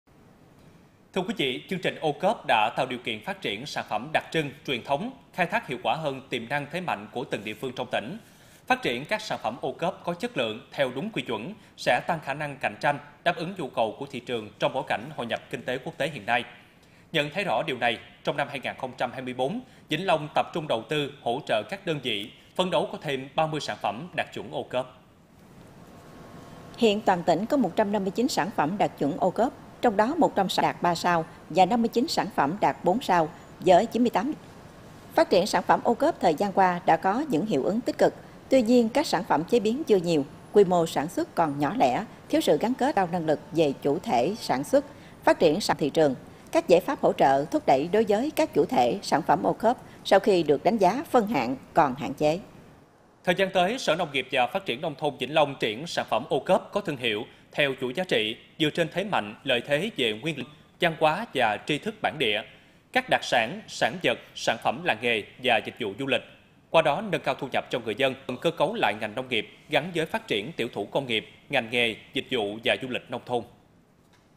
Vĩnh Long phấn đấu có thêm 30 sản phẩm đạt chuẩn OCOP
Thời gian tới, Sở Nông nghiệp và Phát triển nông thôn Vĩnh Long tiếp tục định hướng phát triển sản phẩm OCOP có thương hiệu, theo chuỗi giá trị, dựa trên thế mạnh, lợi thế về nguyên liệu địa phương, văn hóa và tri thức bản địa; các đặc sản, sản vật, sản phẩm làng nghề và dịch vụ du lịch. Qua đó, nâng cao thu nhập cho người dân; góp phần cơ cấu lại ngành nông nghiệp gắn với phát triển tiểu thủ công nghiệp, ngành nghề, dịch vụ và du lịch nông thôn

⬇️⬇️⬇️ Tải THVL Audio để nghe đầy đủ